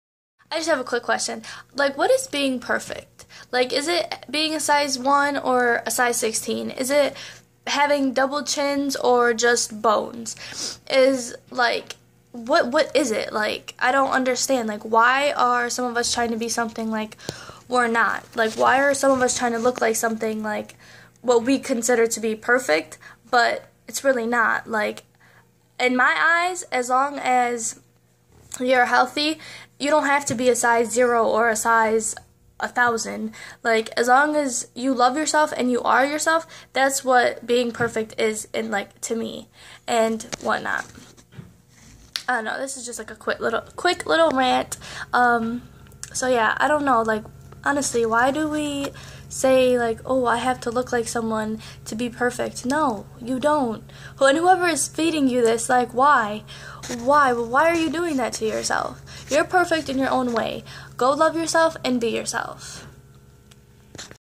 Rant: What Is Perfect?
Being yourself and ONLY yourself is perfect. DON'T be like everyone else. :)